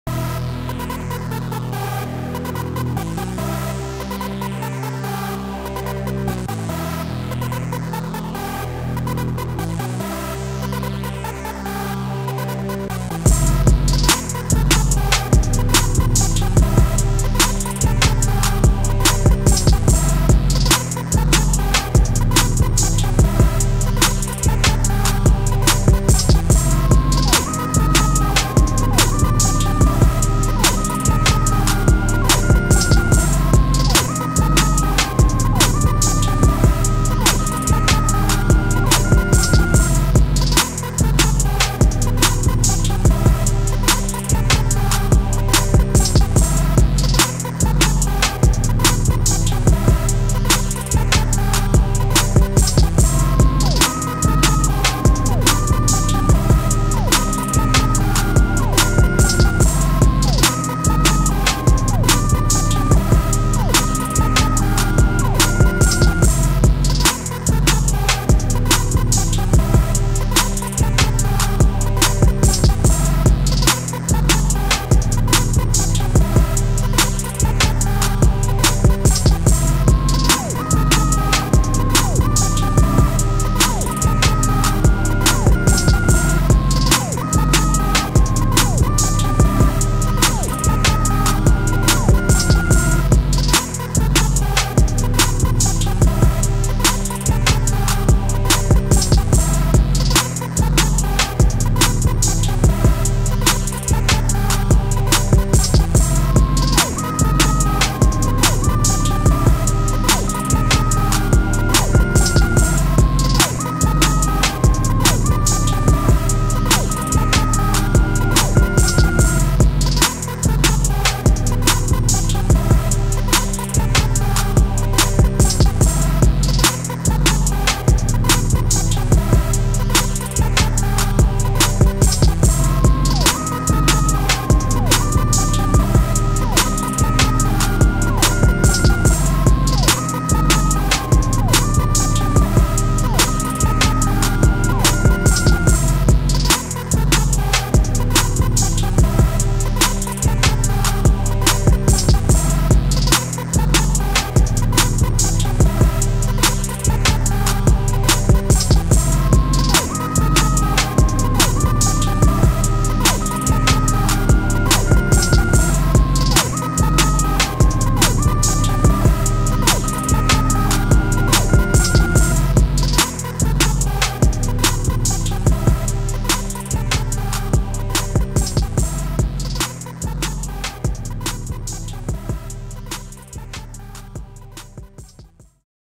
kc . productions production studio hiphop beat mix 145 tempo beat mix.2021
kc . productions studio  hiphop beat mix 145 tempo beat mix.2021
producer artis
instruments
mellowe synth wav instrument
saturation 5th wav instrument
slow synth wav instrument
hiphop drums 145 tempo
producer kc.productions
record label kc.productions